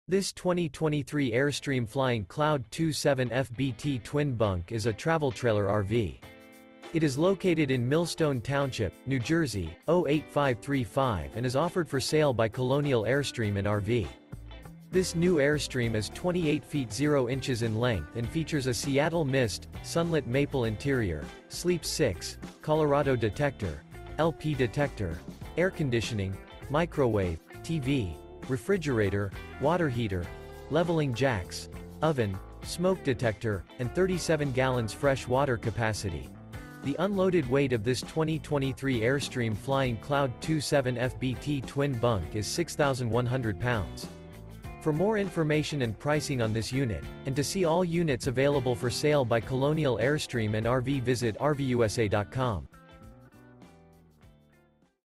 Spectacular 2023 Airstream Flying Cloud Travel Trailer RV For Sale in Millstone Township, NJ
This 2023 Airstream Flying Cloud 27FBT Twin Bunk is a Travel Trailer RV. It is located in Millstone Township, NJ 08535 and is offered for sale by Colonial Airstream & RV. This New Airstream Is 28' 0" in length and features a Seattle Mist/Sunlit Maple interior, sleeps 6, Water Heater, Oven, Awning, Air Conditioning, External Shower, DVD Player, LP Detector, TV, Smoke Detector, CO Detector, and 37 gal fresh water capacity.The unloaded weight of this 2023 Airstream Flying Cloud 27FBT Twin Bunk is 6100 lbs.